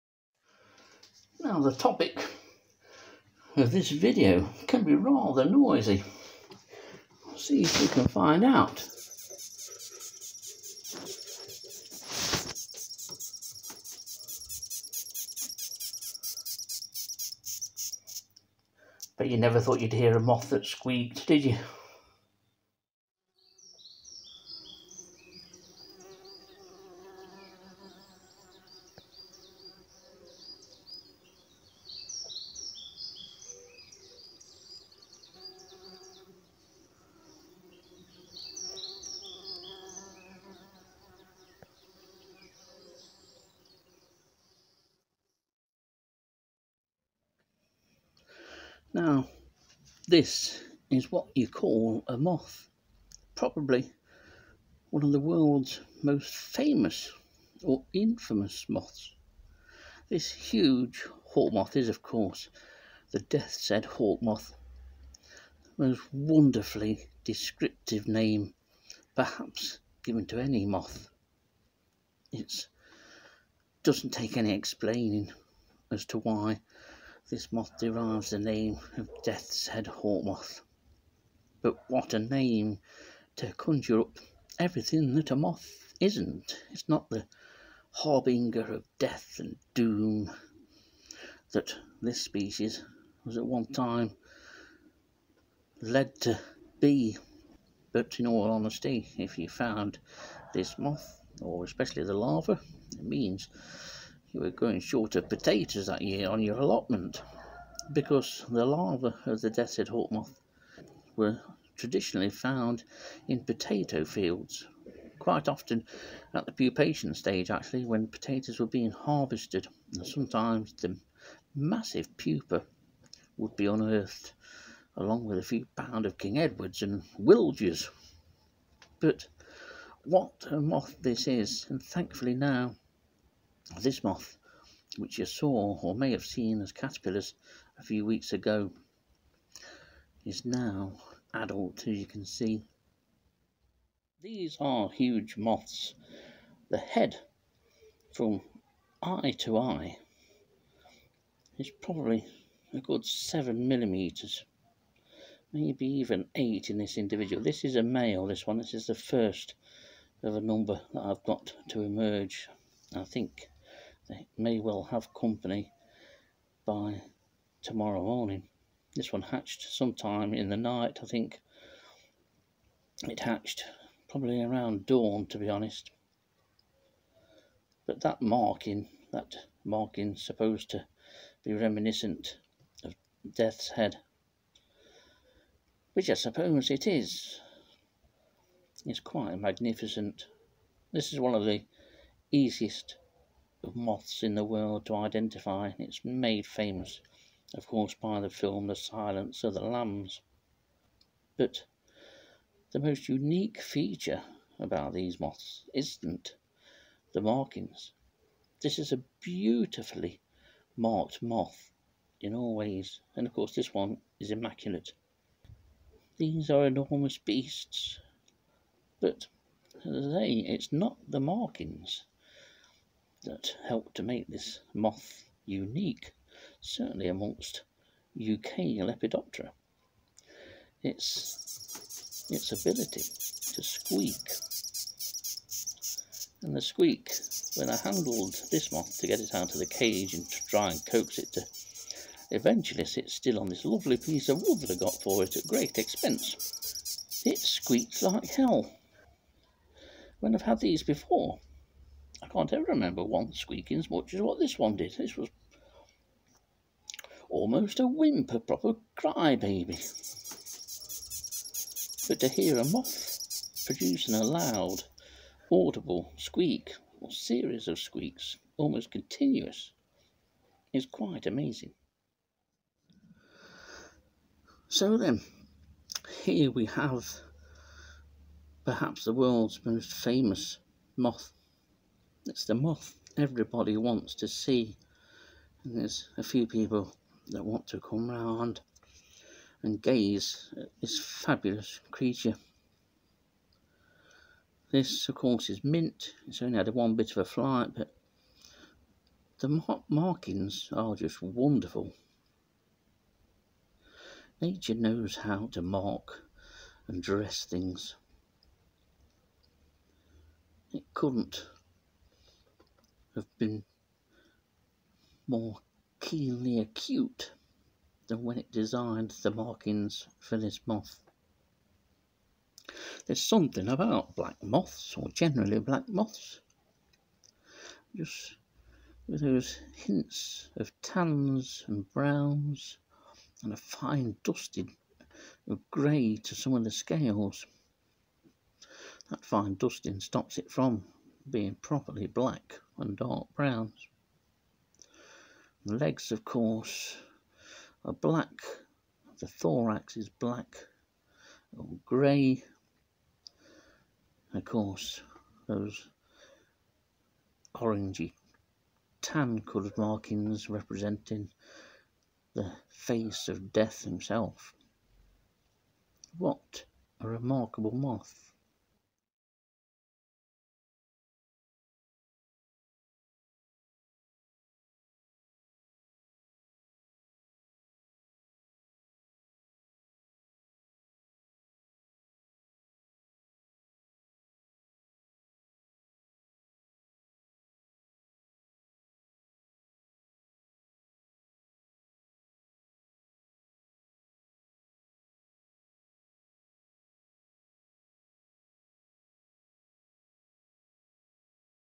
The moth that squeaks - the Death's head Hawk moth
After rearing the wonderfully large larvae, its time to have a look at the extremely impressive adult moth. And it turned out to be noisy as well!